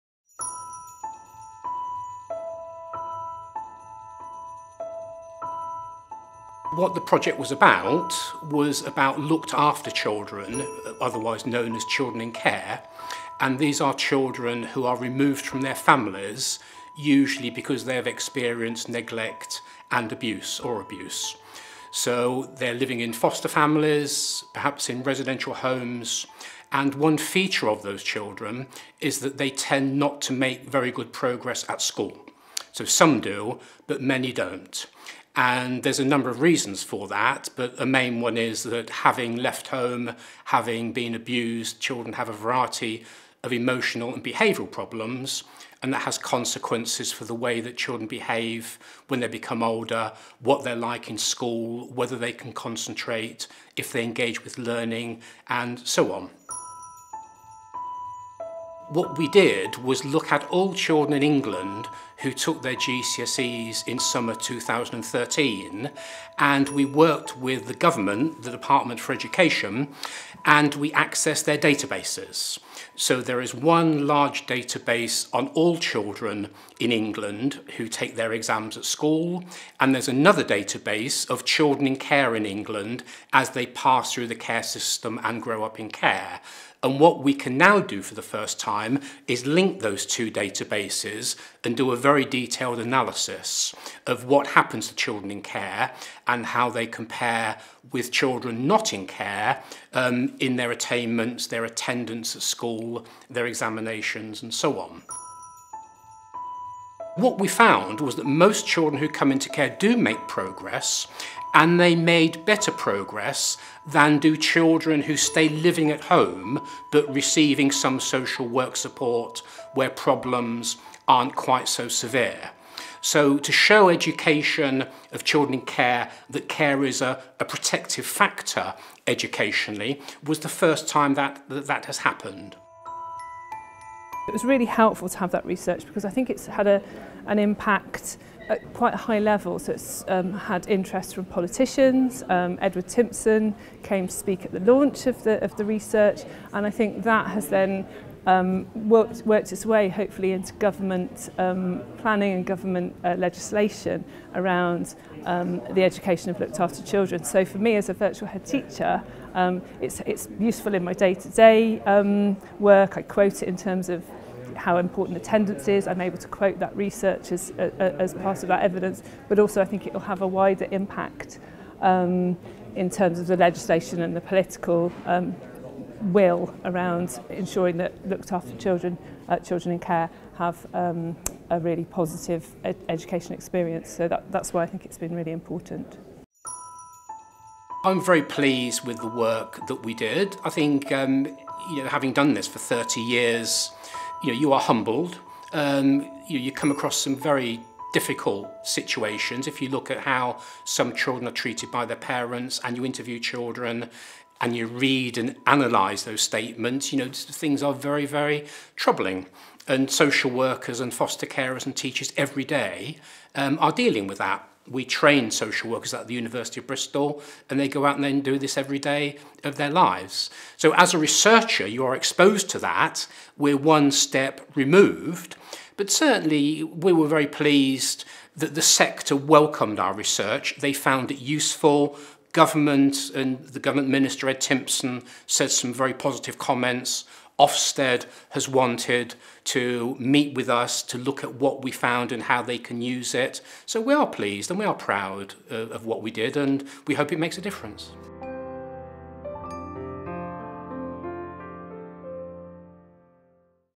Professor David Berridge, Children in Care
Research comparing data sets gives insights into the progress made by children in Care.  Winner of the 2015/16 University of Bristol Vice Chancellor's Impact award, Policy and Practice.